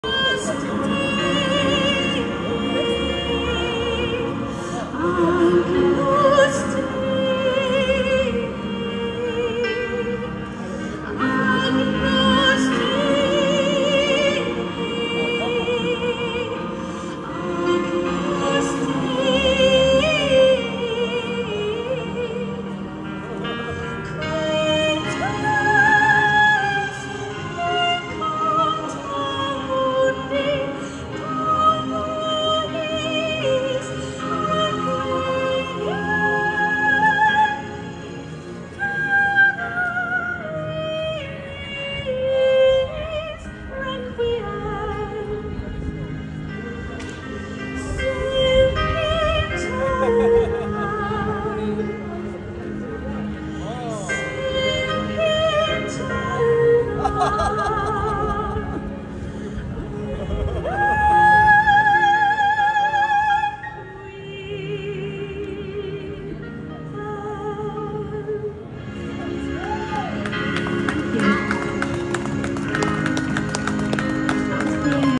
Edinburgh Festival 22 August 2024  - Chloebeth a trained Opera Singer from Yorkshire